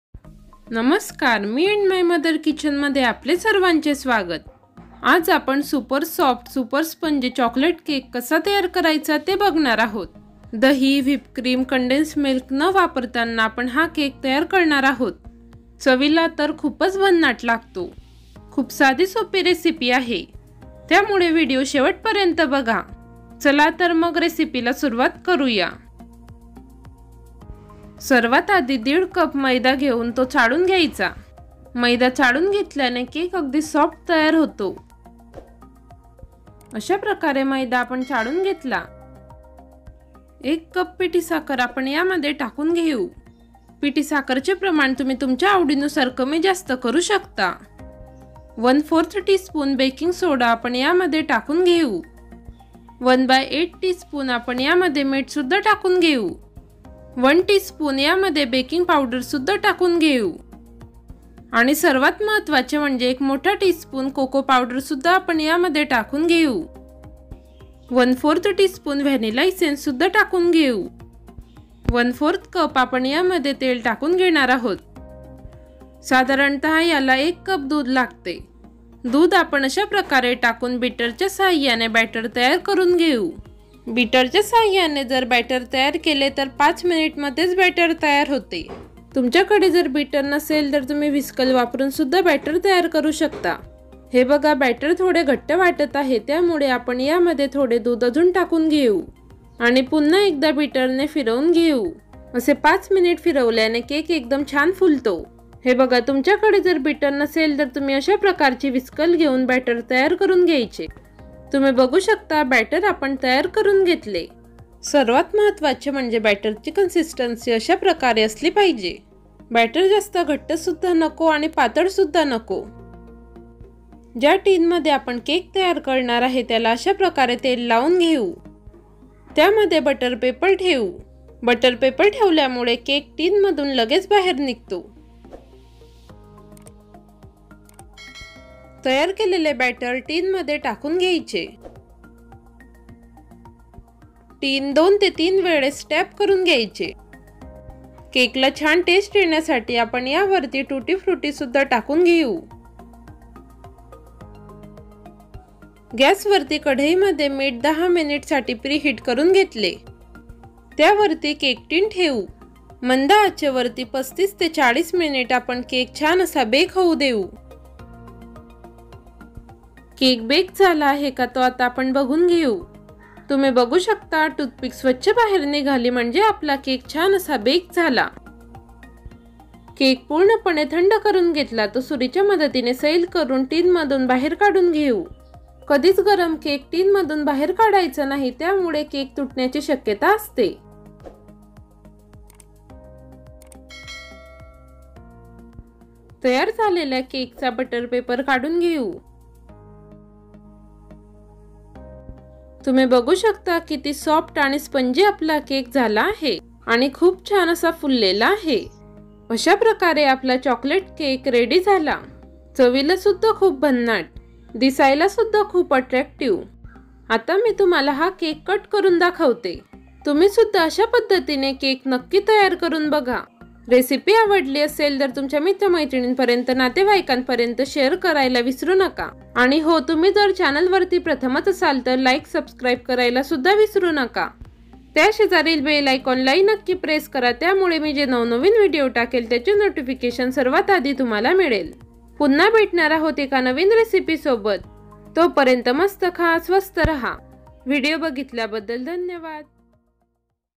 Chocolate Cake Recipe | Eggless Cake | Soft and Sponge Chocolate Cake | Easy Cake | चाॅकलेट केक
साहित्य:-
मैदा - 1 1/2 cup
पिठी साखर - 1 cup
बेकिंग पावडर - 1 tsp
बेकिंग सोडा - 1/4 tsp
मीठ - 1/8 tsp
कोको पावडर - 1 tsp
तेल - 1/4 cup
दूध - 1 cup
Tutti Frutti